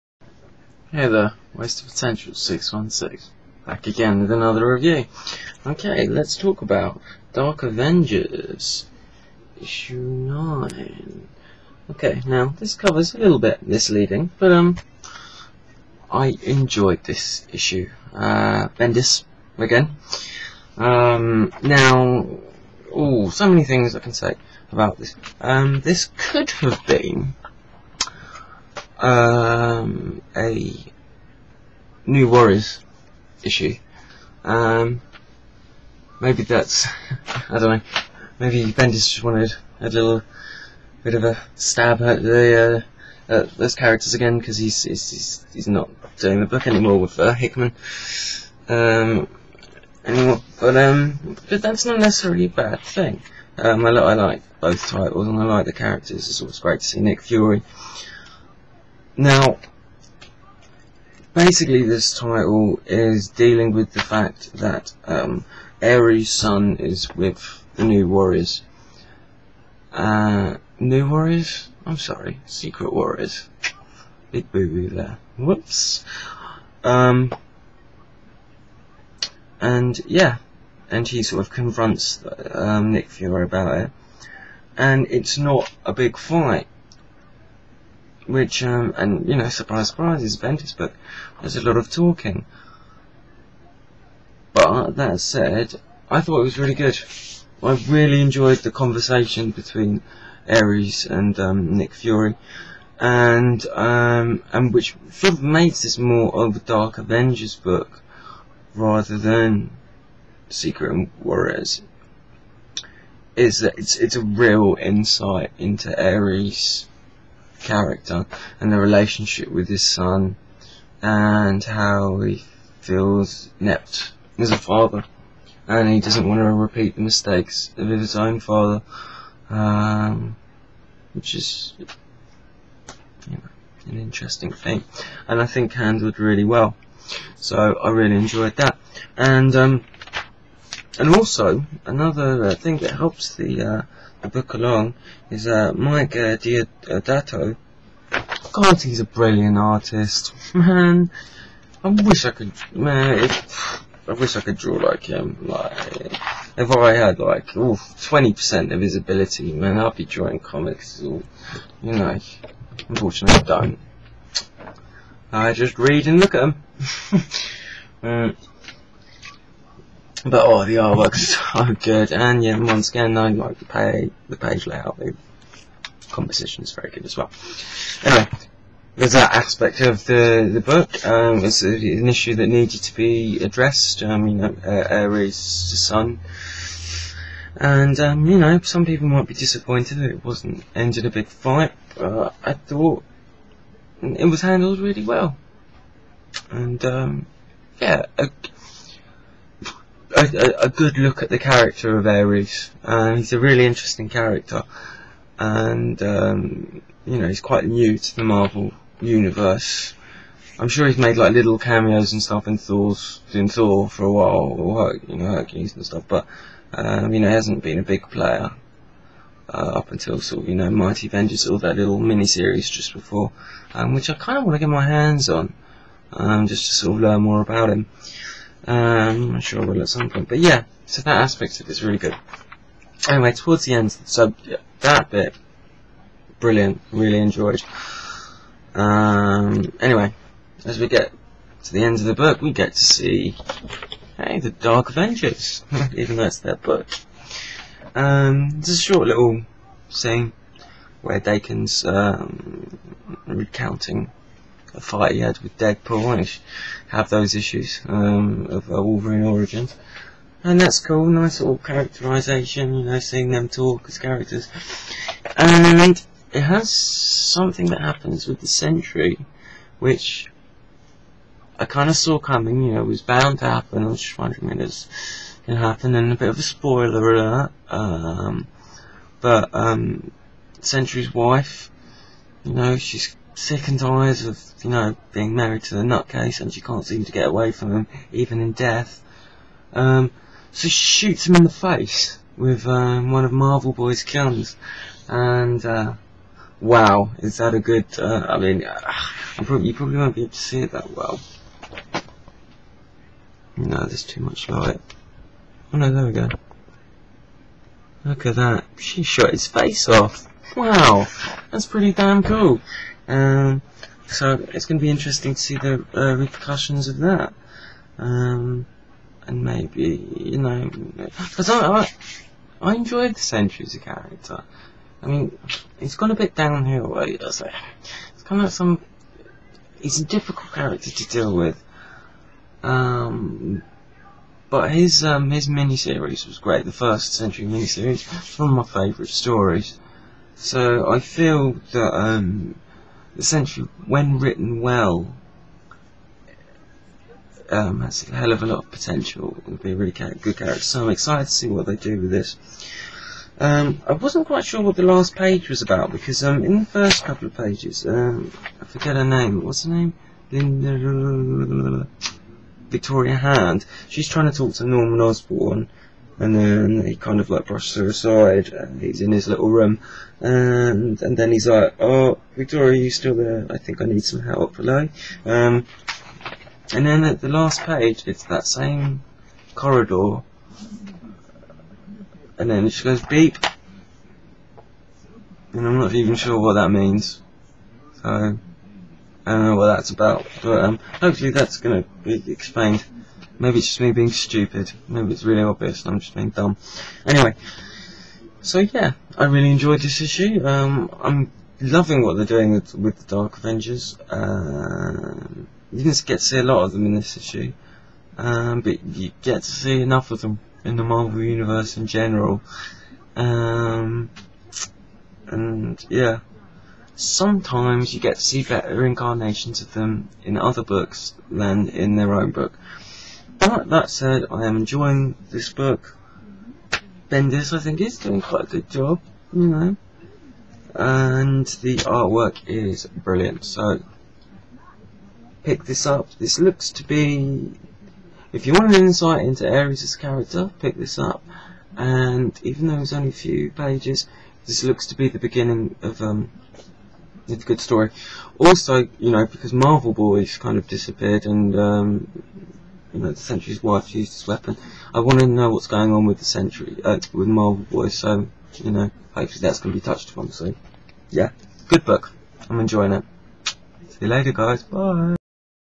WP616 Dark Avengers issue 9 comic review
Dark Avenger's Ares meets the Secret Warriors over the fate of his son,and some last minuate Sentry crazyness..reviewed